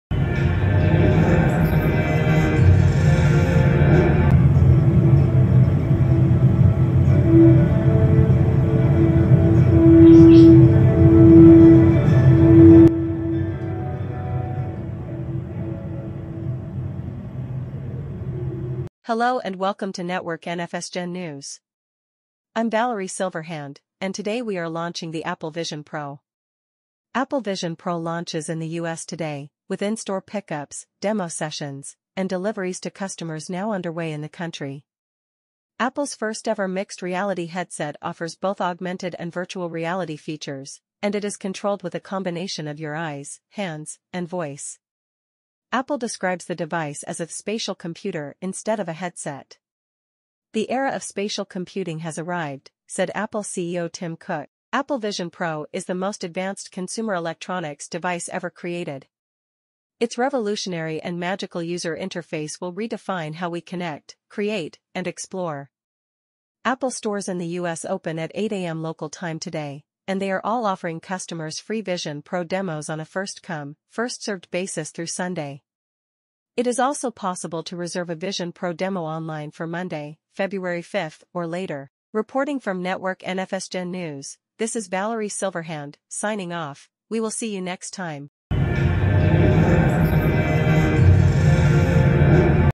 Apple Vision Pro Launches Today | Network NFSGen News
Apple Vision Pro launches in the U.S. today, with in-store pickups, demo sessions, and deliveries to customers now underway in the country.